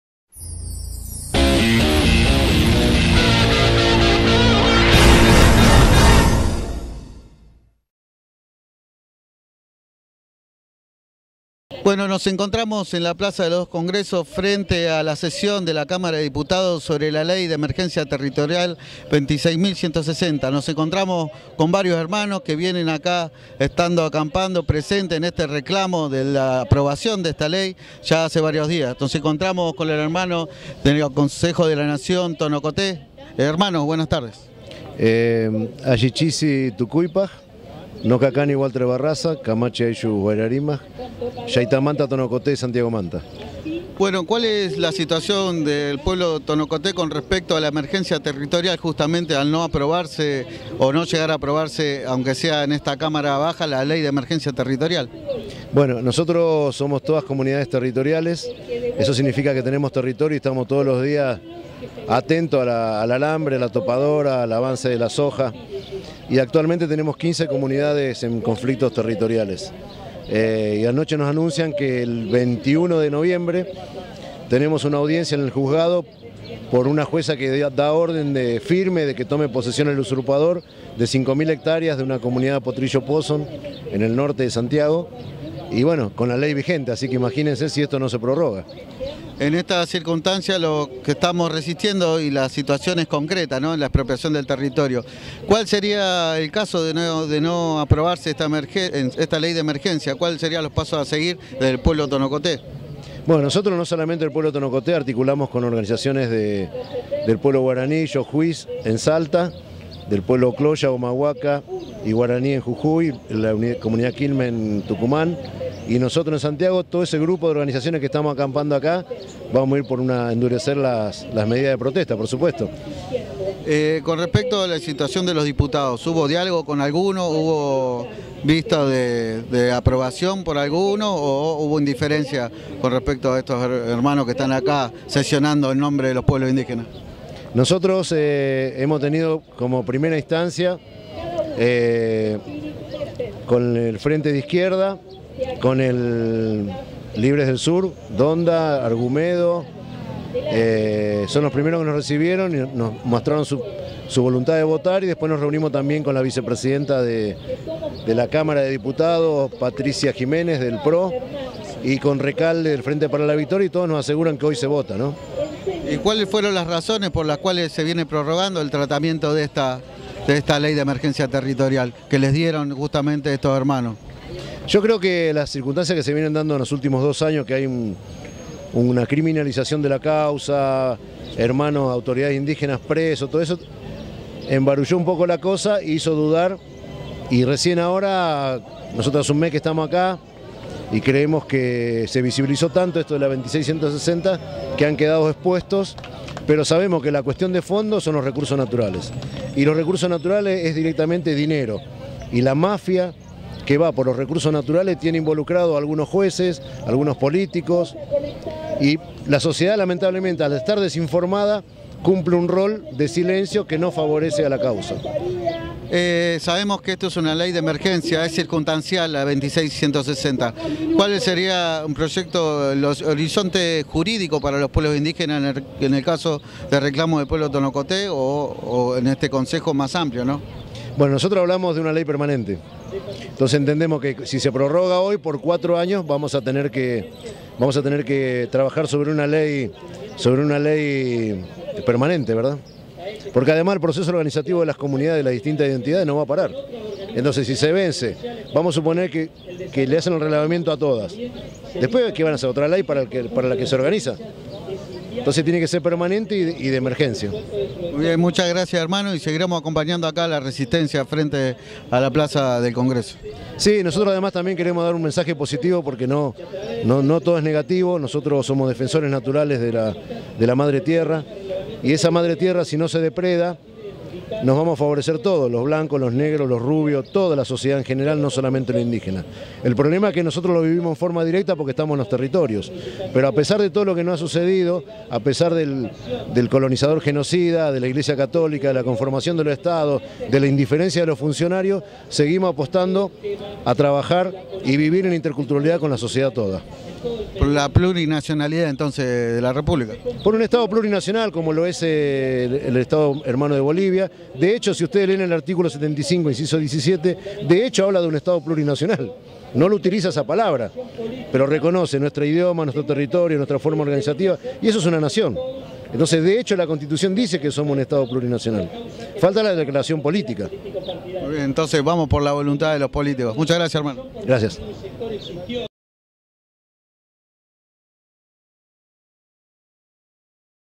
Manifestación popular a favor de prorroga Ley 26160, Hugo Barraza Referente Tonokote
8 de noviembre, Plaza de los dos Congresos, CABA

Manifestación popular a favor de prorroga Ley 26160.
Durante la votación en la Cámara de Diputados de la Nación por la prorroga de la Ley 26160 (sancionada en 2006 y publicitada como parte de la “reparación histórica” para los pueblos originarios para el freno a los desalojos y censo catastral de territorios indígenas), miles de ciudadanos de diferentes organizaciones politicos y sociales se manifestaron pacificamente en apoyo a la prorroga de la mencionada Ley.
Hugo Barraza, representante del Consejo de la NaciónTonokote nos cuenta como es la situación actual con el pueblo ancestral y el avance de las multinacionales y los estancieros por los territorios en busca de recursos naturales.

TV Sindical, canal 29.2 de TDA CABA

Reportero: Eliseo Alvarez
Cámara y edición: Gabriel Ocampo